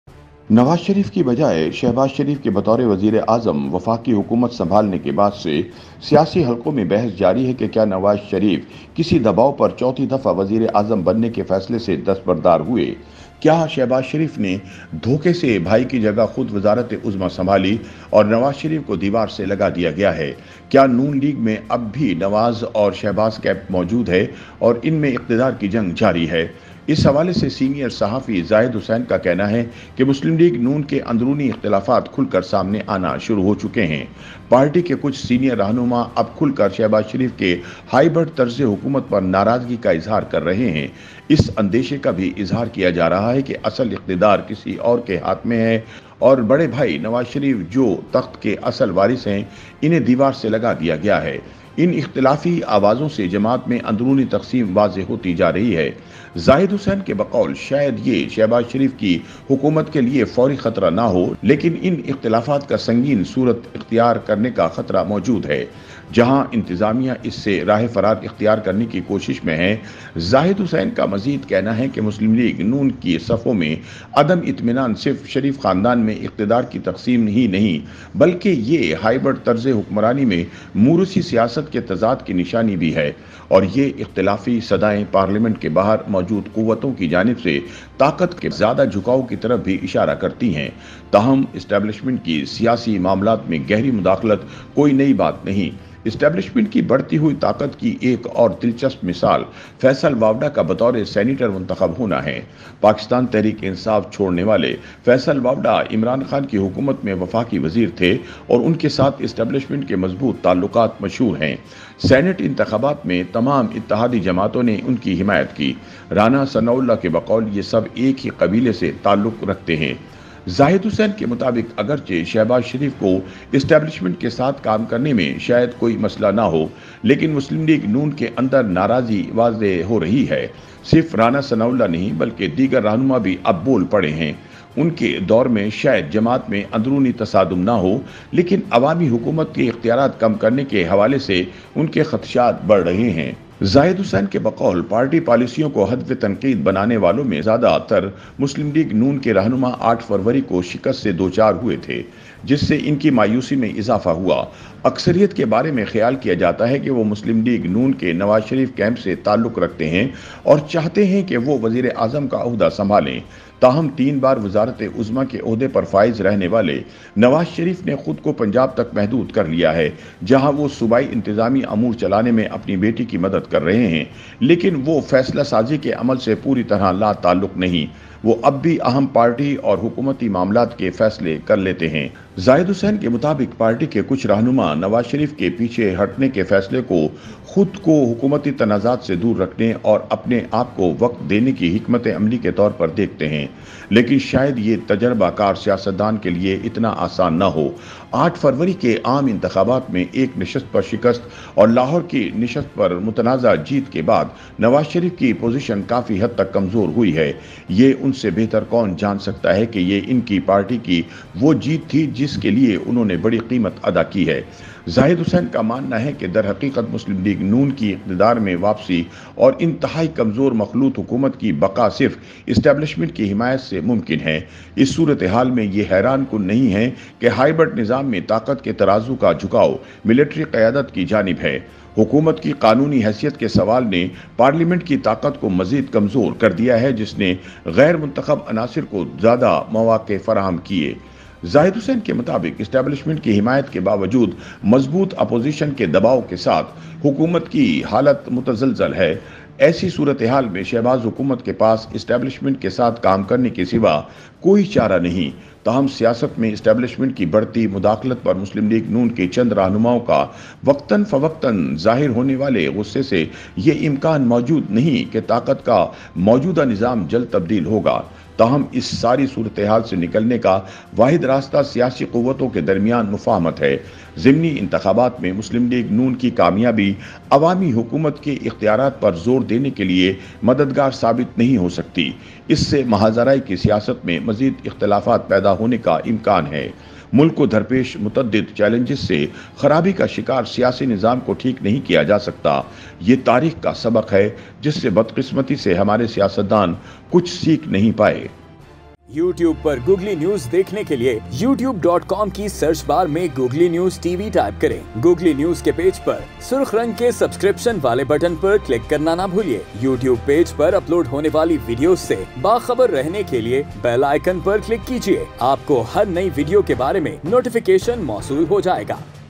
Kya Nawaz Sharif Ka 4th Time PM Ban-nay Ka Khawab Shahbaz Sharif Nai Chakna-Chur Kiya?|Googly NewsTV
Kya Nawaz Sharif Ka 4th Time PM Ban-nay Ka Khawab Shahbaz Sharif Nai Chakna-Chur Kiya?